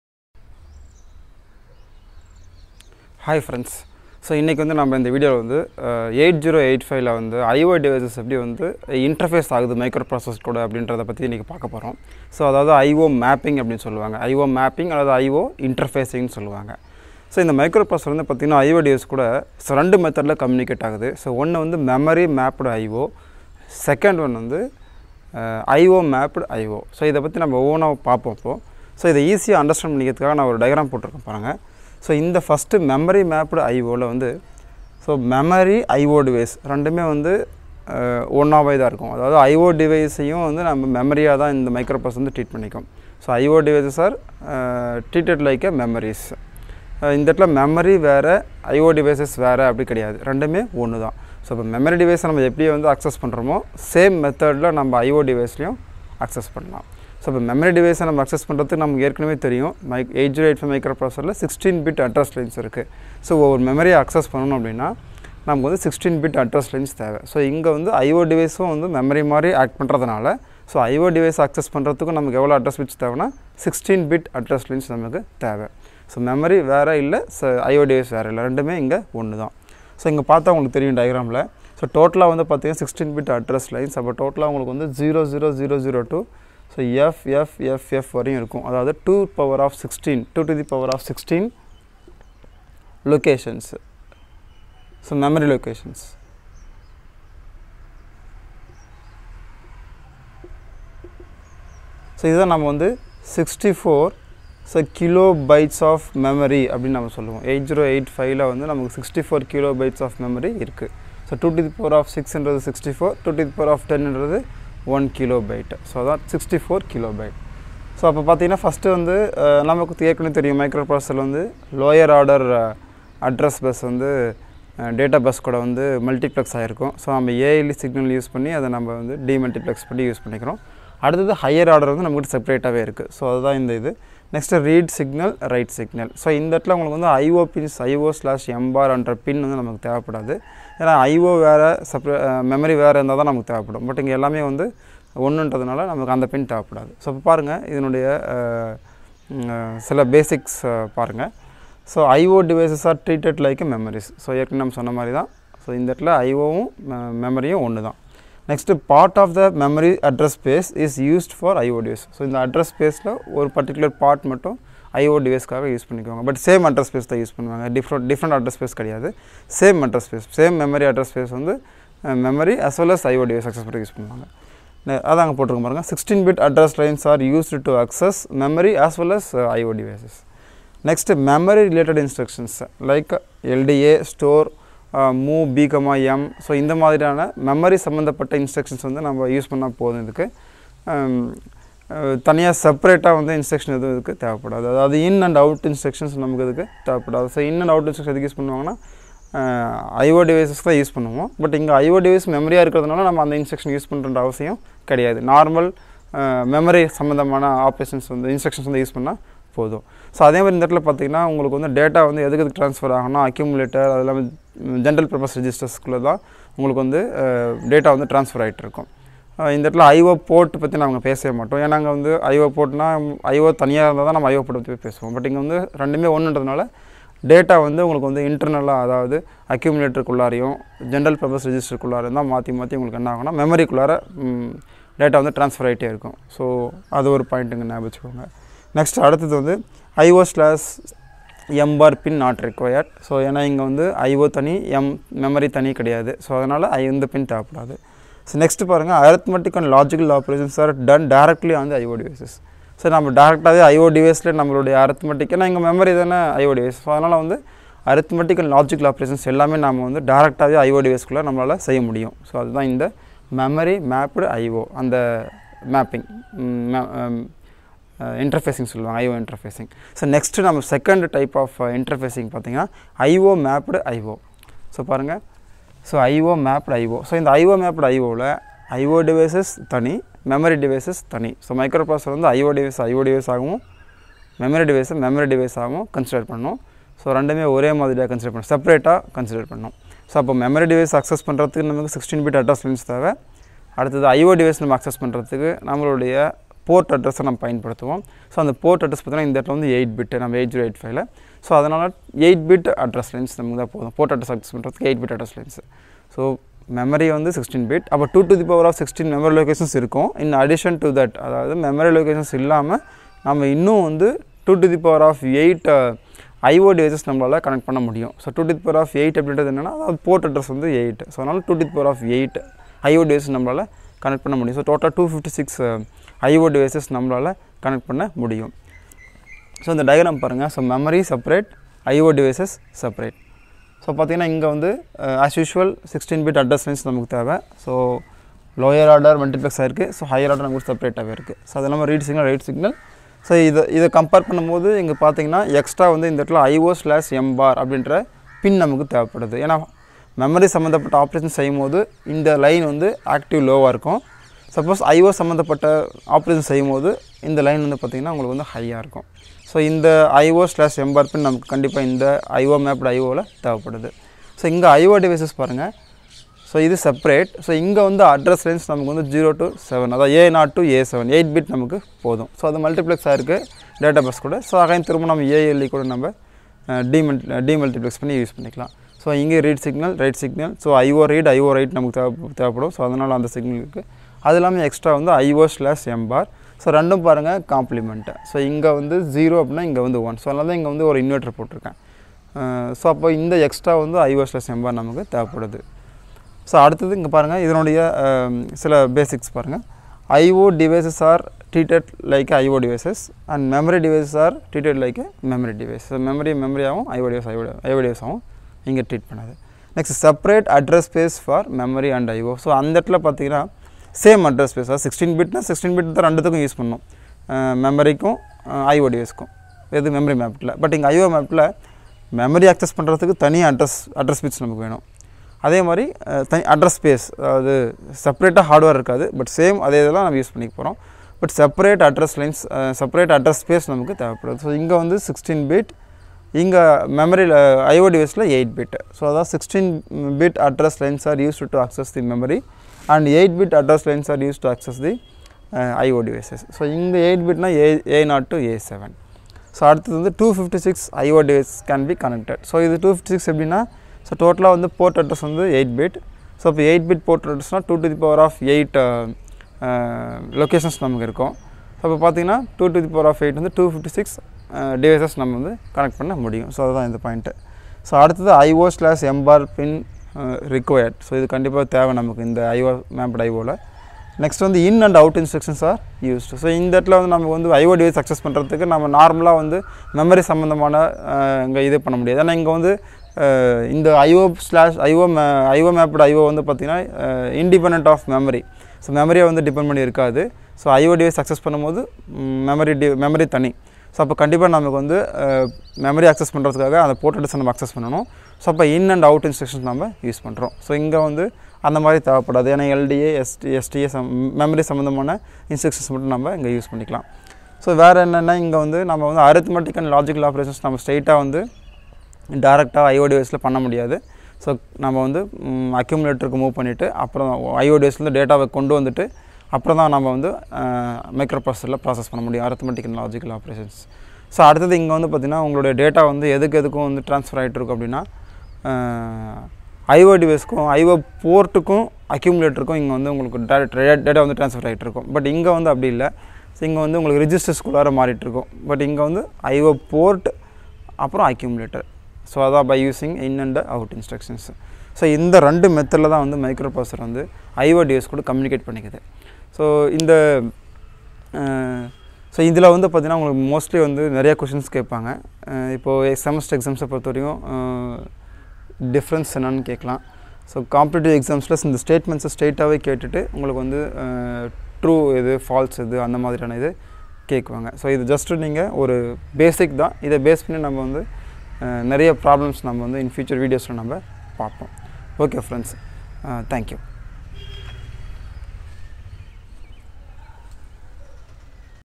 Interfacing with 8085 microprocessor | Memory mapped I/O and I/O mapped I/O in Tamil
8085 microprocessor communicates with I/O devices in two modes 1.Memory mapped I/O 2. I/O mapped I/O.

This video will help you to understand the two methods of I/O interfacing with 8085 microprocessor.